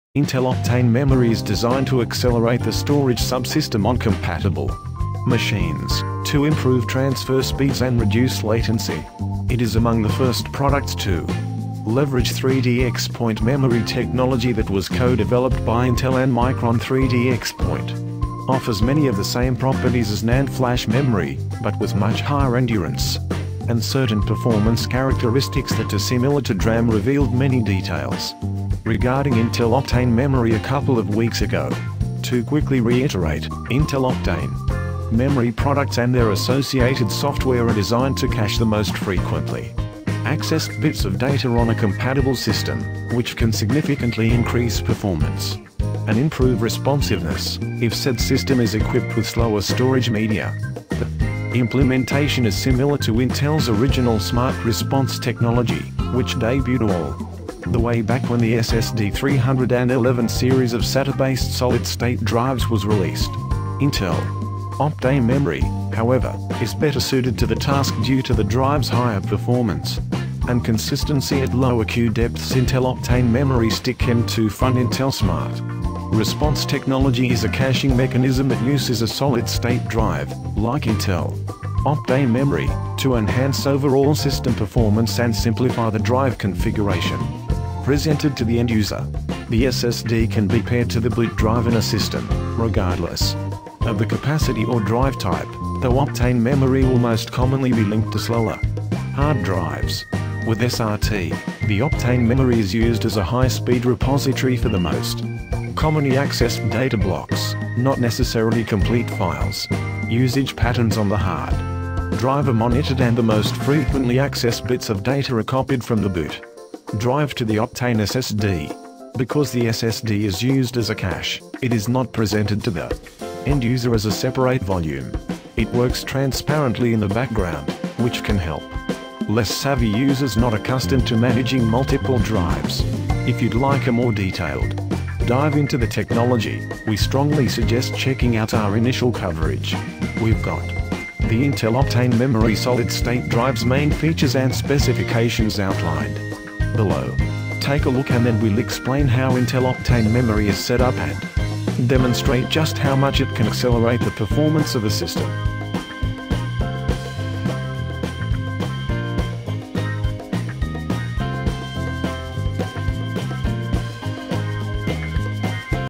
INTEL OPTANE - [FULL REVIEW]
In this video i wanna tell you about intel SSD the short note is given below:
This technology is a unique combination of 3D XPoint™ memory media, Intel Memory and Storage Controllers, Intel Interconnect IP and Intel® software. Together these building blocks deliver a revolutionary leap forward in decreasing latency and accelerating systems for workloads demanding large capacity and fast storage.